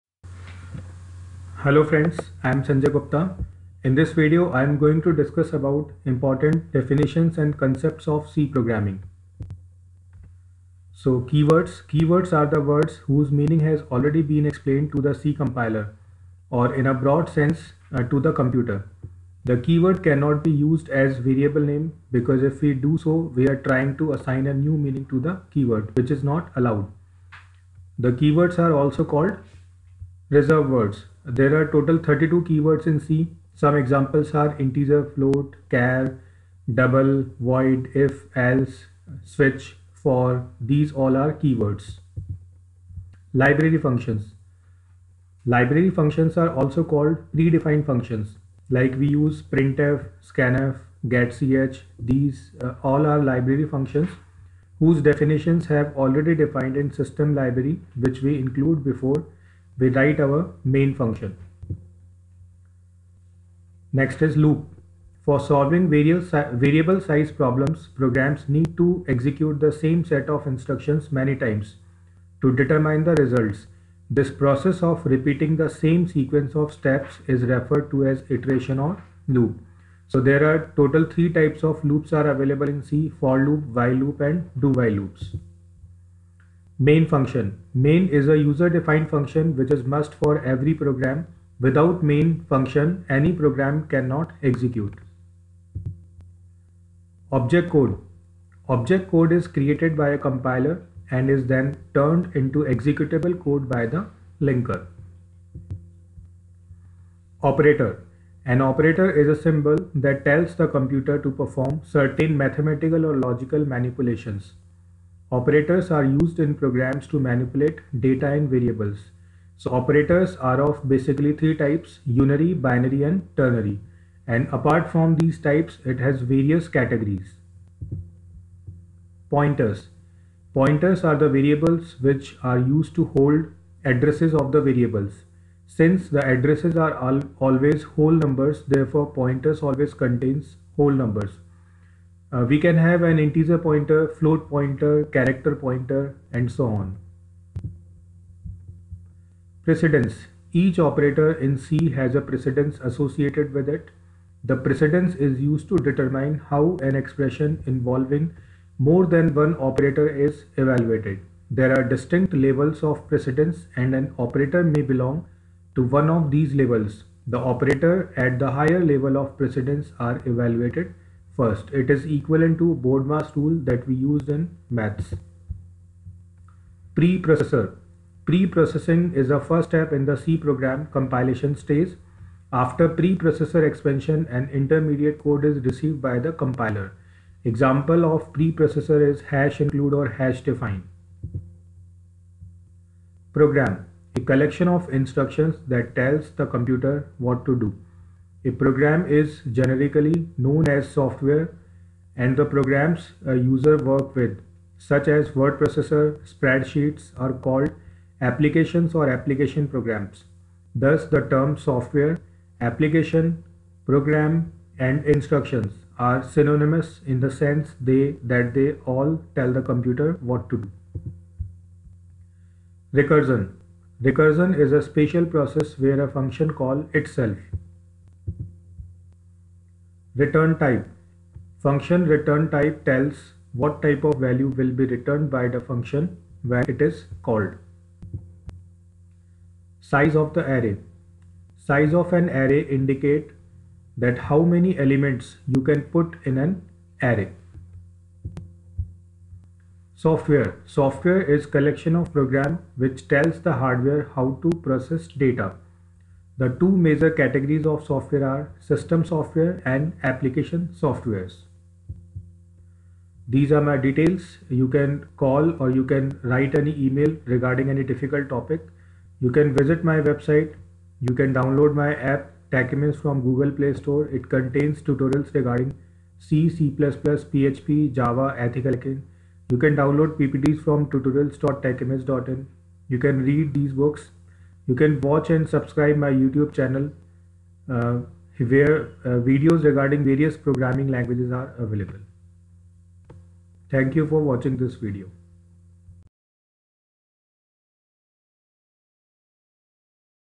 C Programming - Definition - Part 3 - Learn C Language by Sanjay Gupta in English
In this video important definition of c language are discussed. These definitions are important for interview purpose.